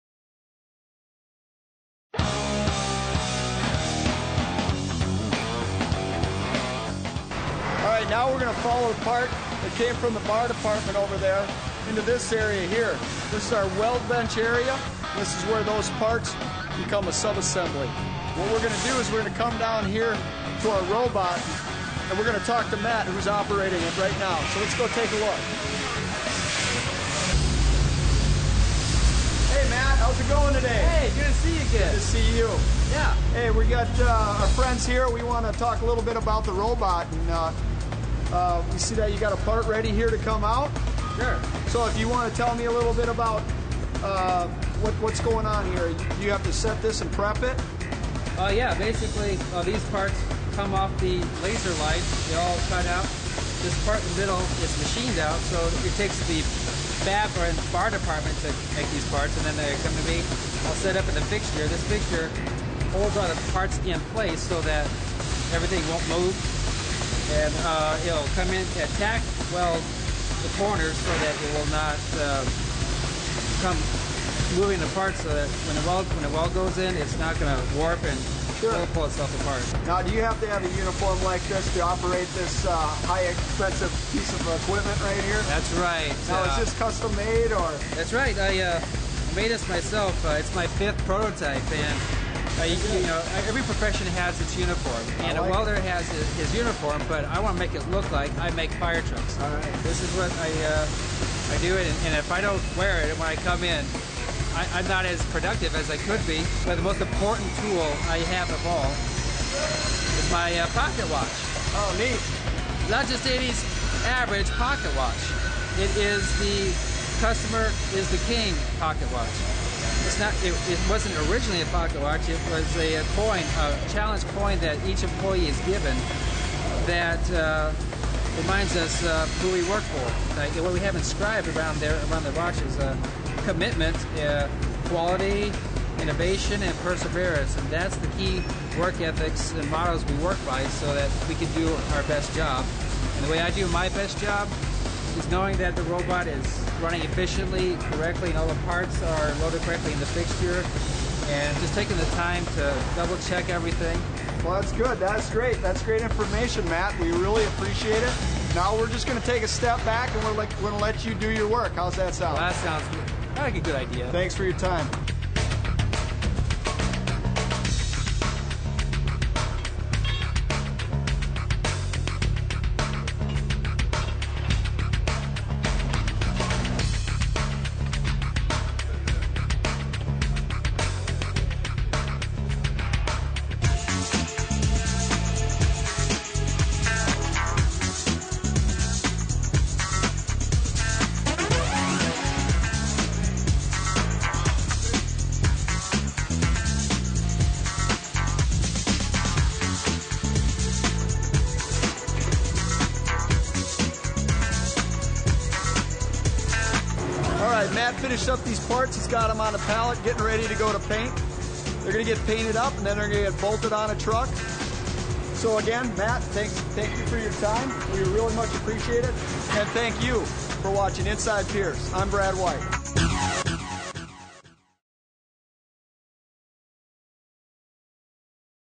Inside Pierce - Weld Booth
Paying attention to every detail is something Pierce strongly believes in.  Watch how human passion combined with robot labor weld your truck.